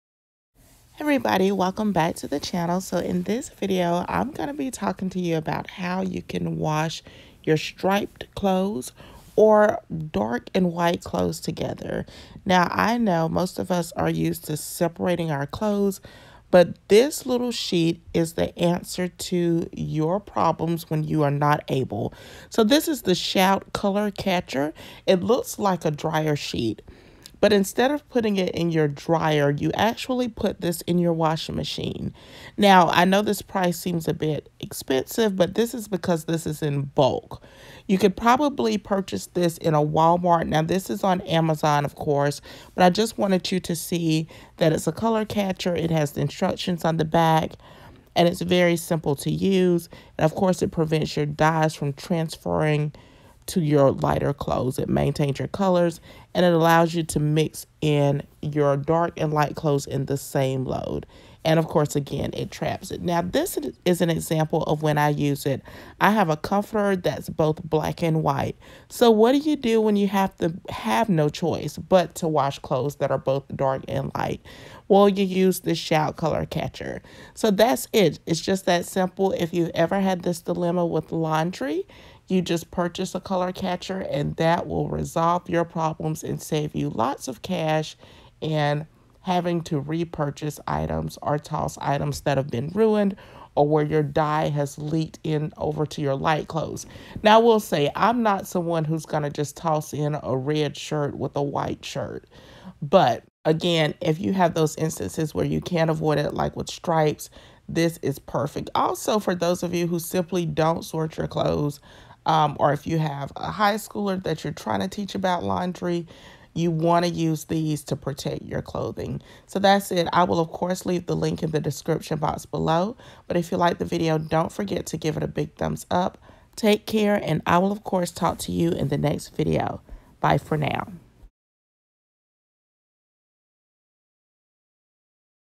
HOW TO WASH MIXED COLORS | COLOR CATCHER LAUNDRY HACK
If you're wondering how to wash mixed colored clothes, to stop color bleed, watch this video. If you're learning how to do laundry for the first time, these laundry solutions will be helpful.  For mixed colors, wash your clothes in cold water, to stop colors from fading.  You can also wash clothes in salt, to keep your colors bright.
Also try Shout color Catcher
These sheets can also help your clothes maintain their original colors.